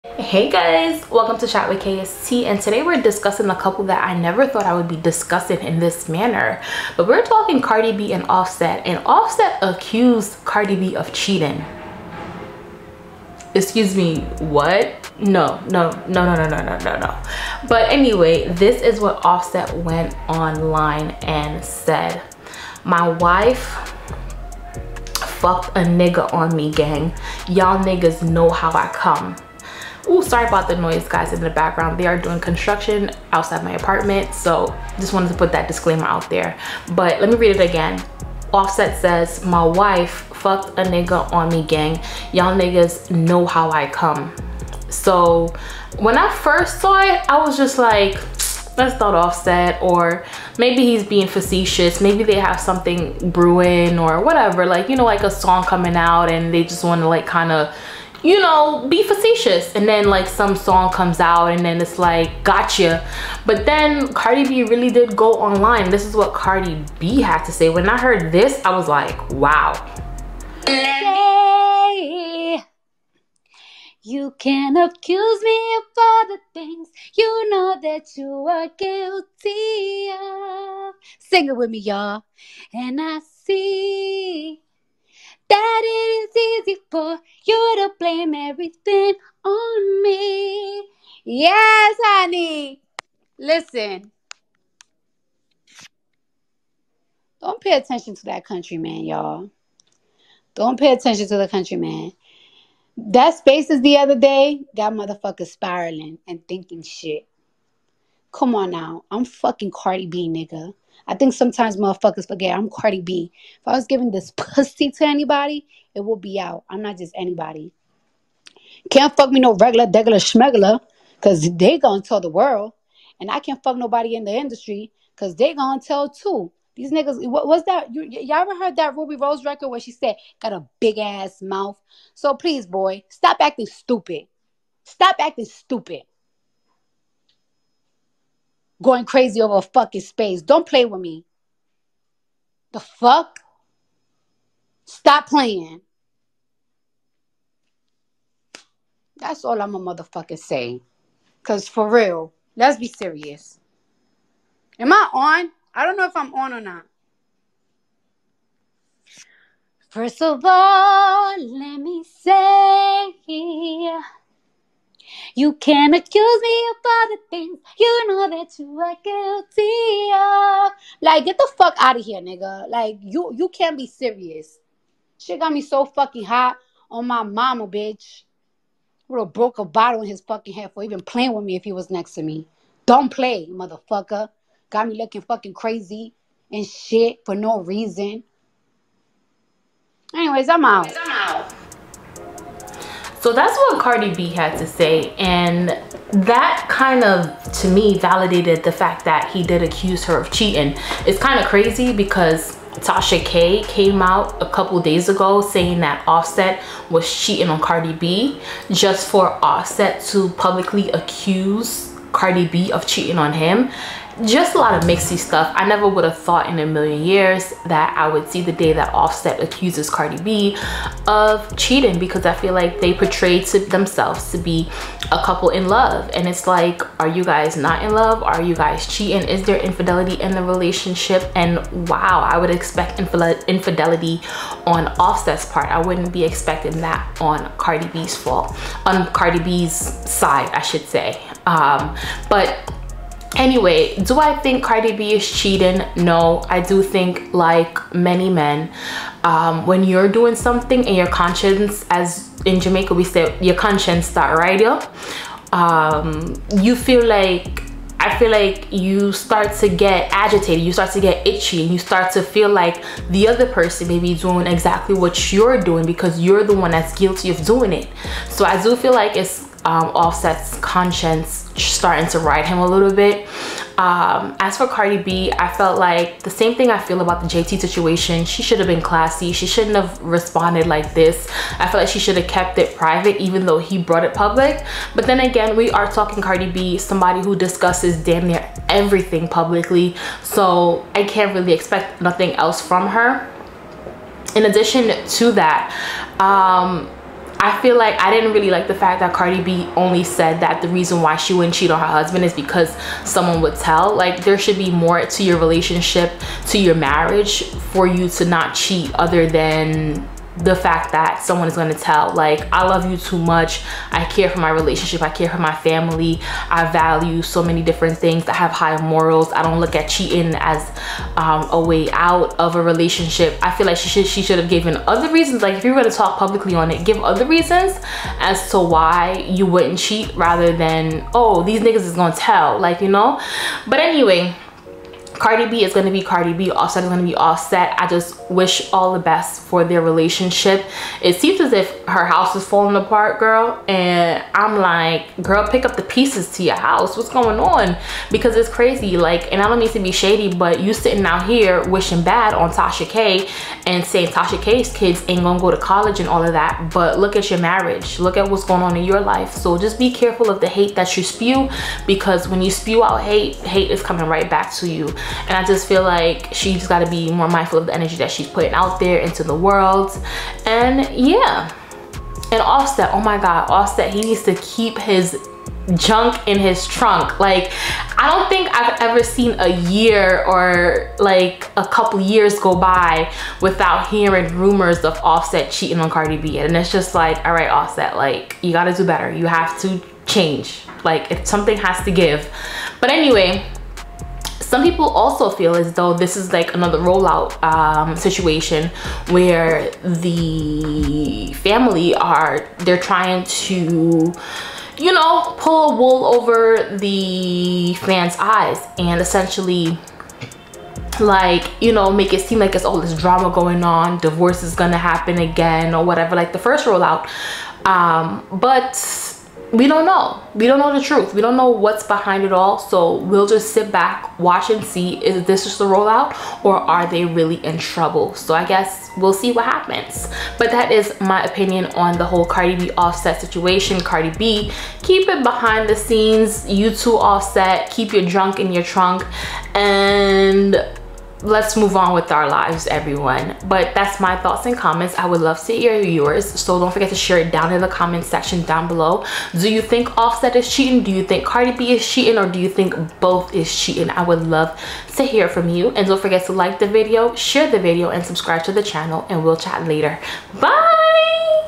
Offset EXPOSES Cardi B For Cheating
Welcome to Chat With Kay Ess Tee,

Today we discuss Cardi B cheating on Offset.